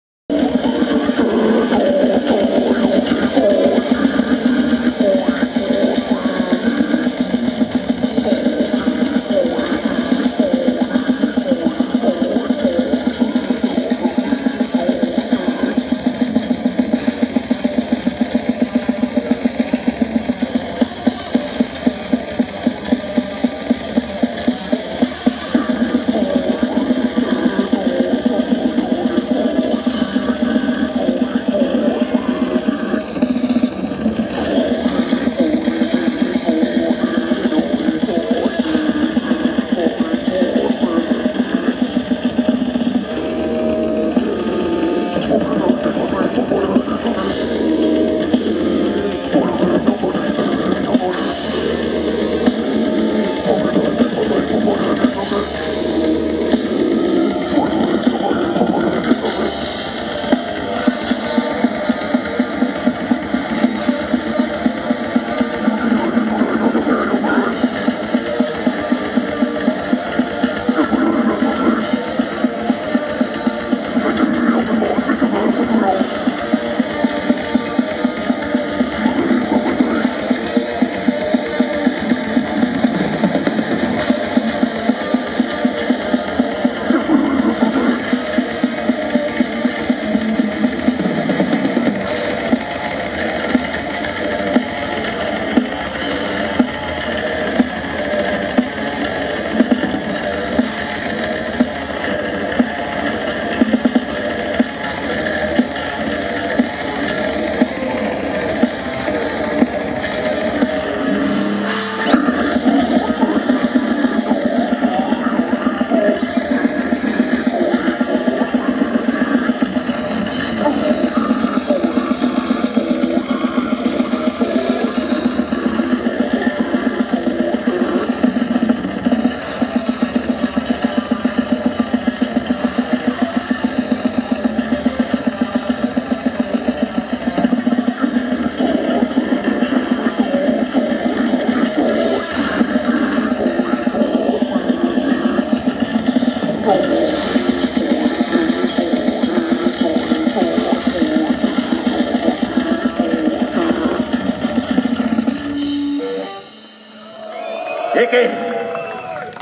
Cranial Carnage part 2/6 live Czech Death Fest vol.8
Červený Kostelec areál Brodský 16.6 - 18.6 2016